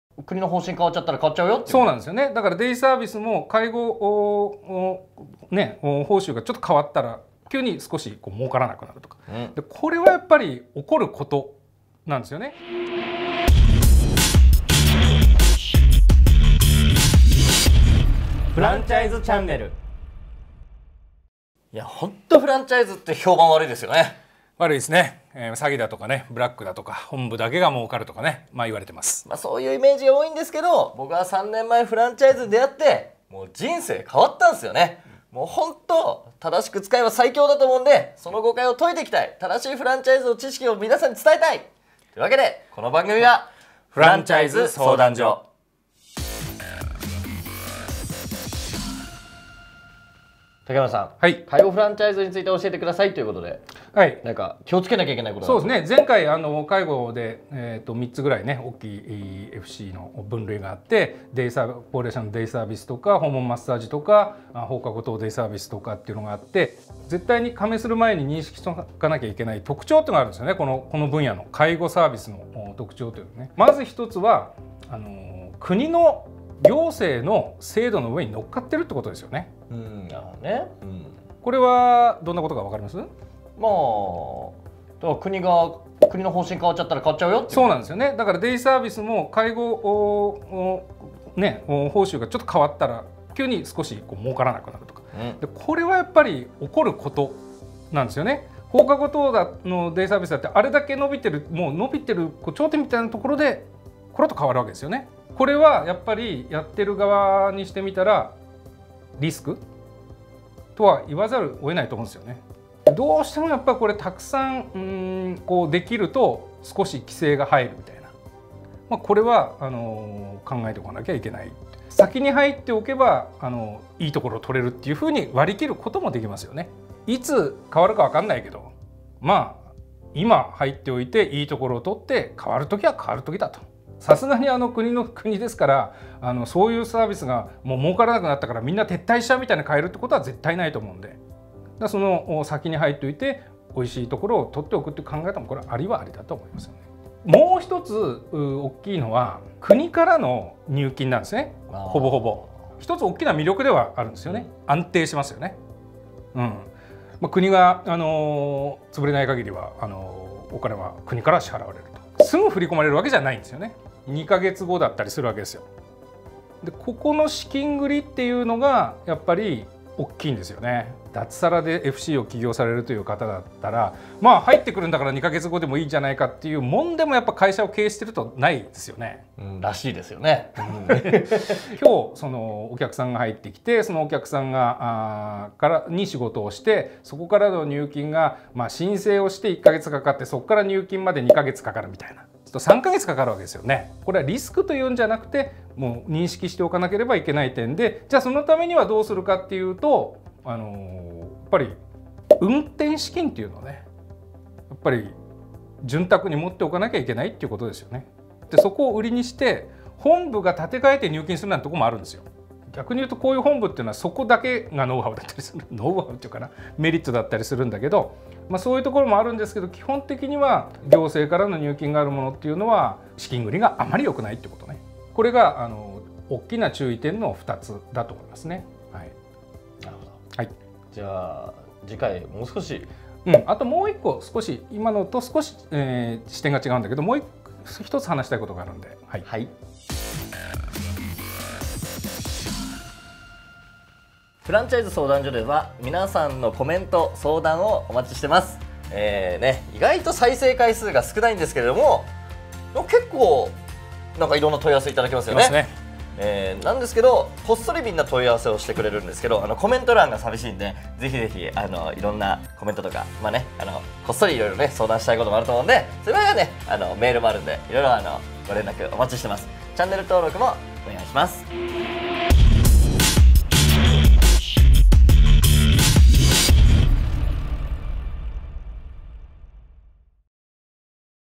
介護(福祉)関係のFCについて教えて！！【中編】｜フランチャイズ相談所 vol.161
フランチャイズのお悩みに動画ですべて答えます！

今回の相談は「介護(福祉)関係のFCについて教えて！！」です！
資金繰りが大変！？
制度が変わったらどうなるの？？

【出演者情報】